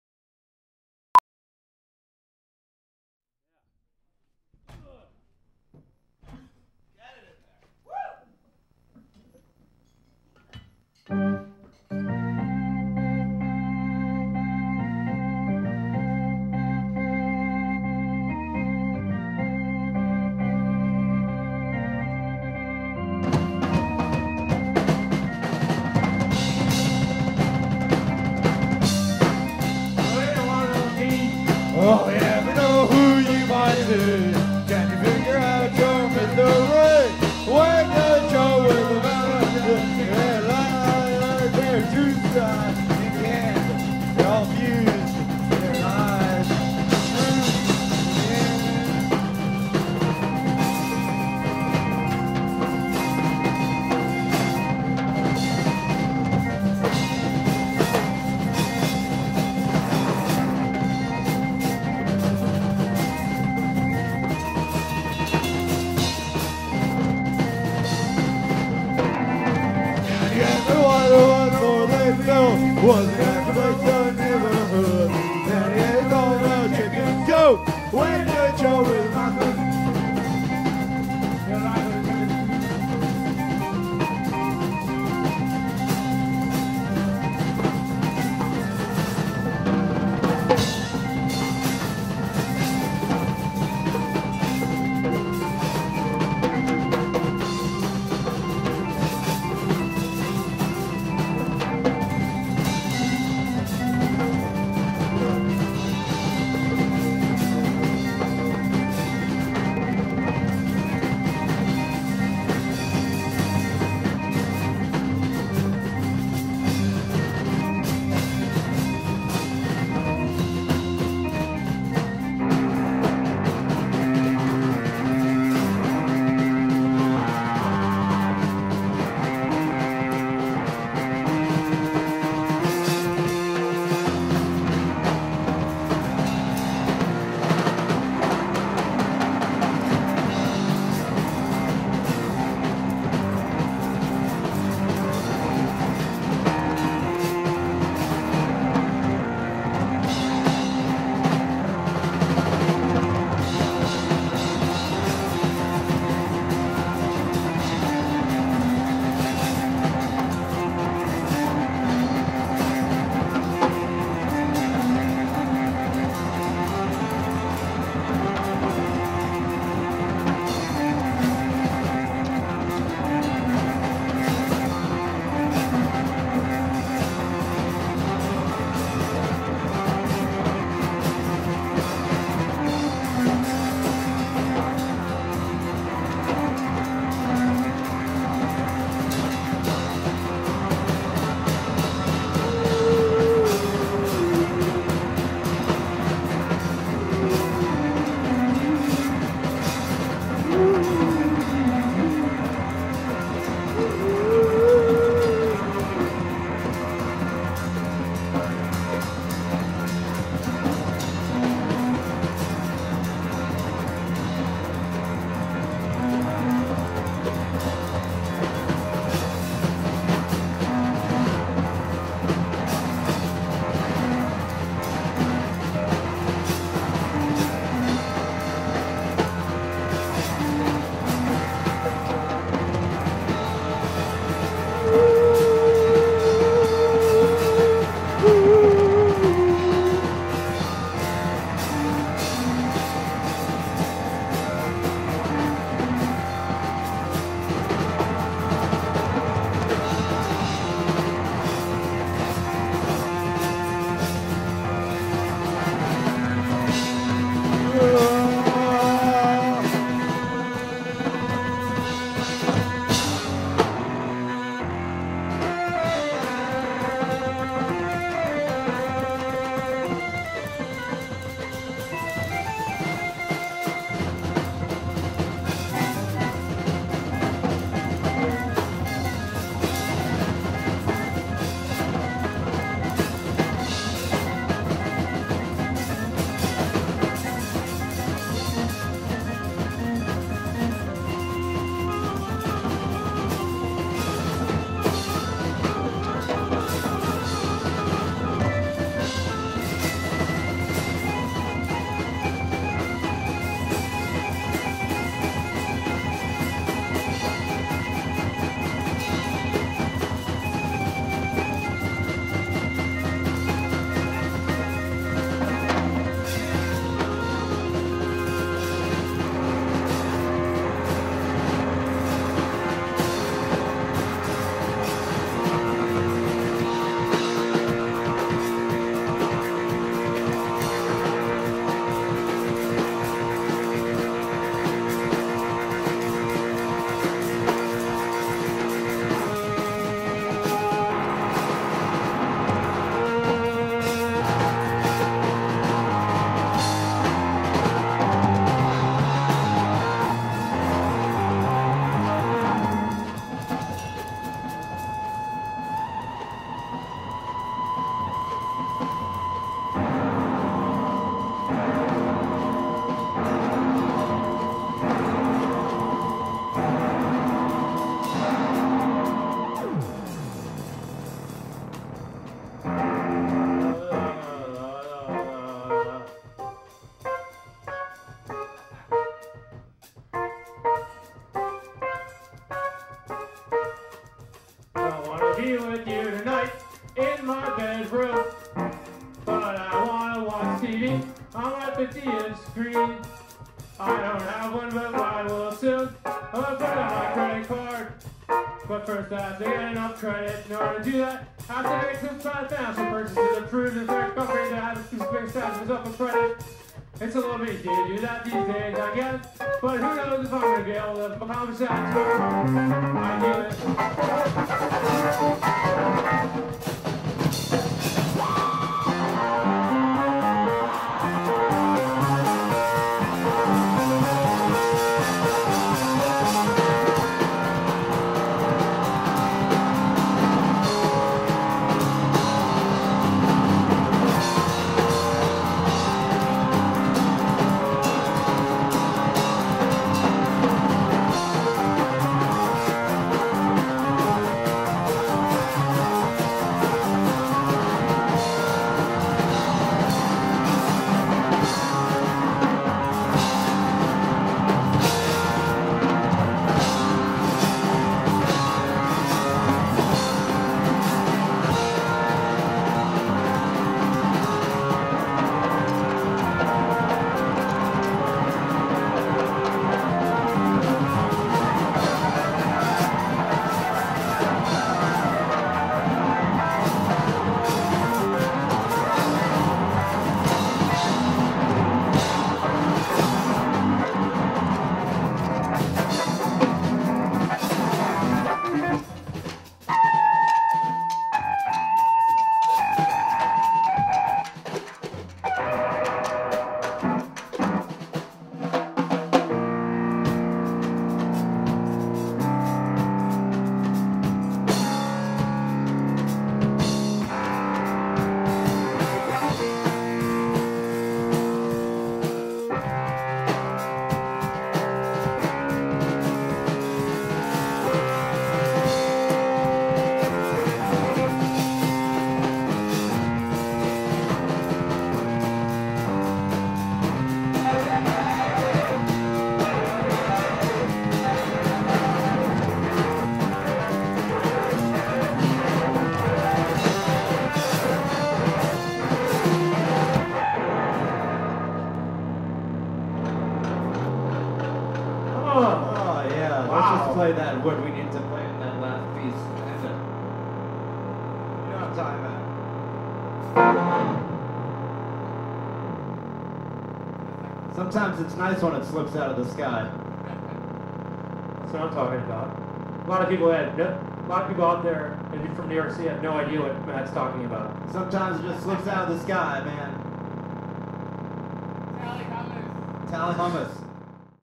TALIBAM! 1-6-09 Bowery Poetry Club, NYC
Talibam! is:
Kevin Shea - Drums
Matt Mottel - Synthesizer

1-6-09 Bowery Poetry Club, NYC

by Robert O'Haire
at straw2gold pictures